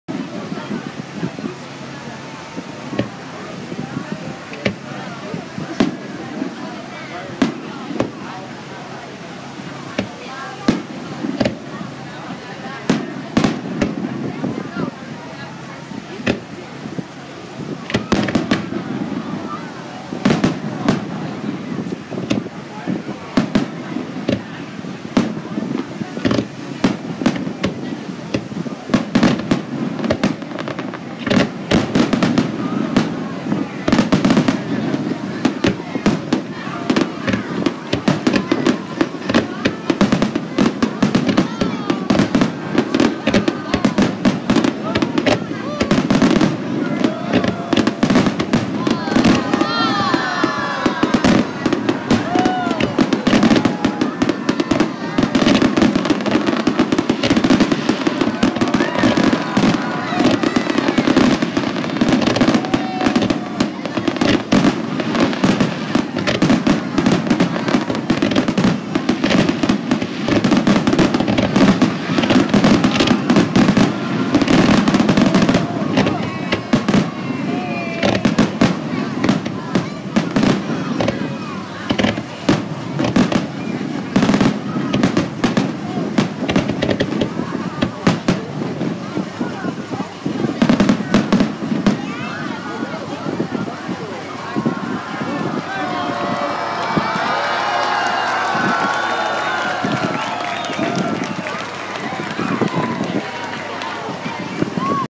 Fireworks revere beach
July 21 2018 fireworks at revere beach after sand sculpture competition